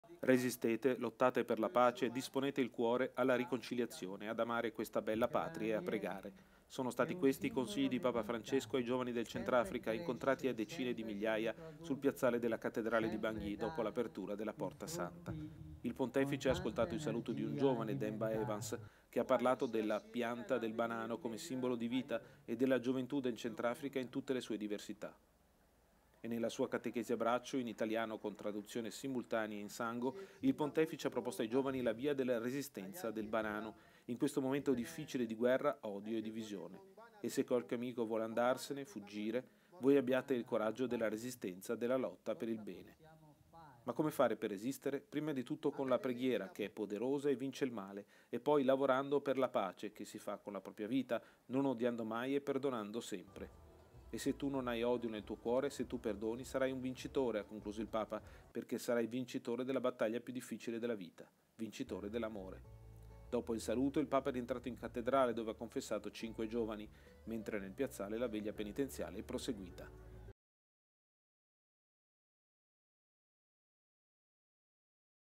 Repubblica Centrafricana, veglia con i giovani 29-11-2015
Resistete, con amore e perdono vincerete

Sul piazzale della cattedrale di Bangui, il Papa ha parlato a migliaia di giovani del Centrafrica di resistenza nel momento difficile di guerra e odio. Dall'inviato Alessandro Di Bussolo.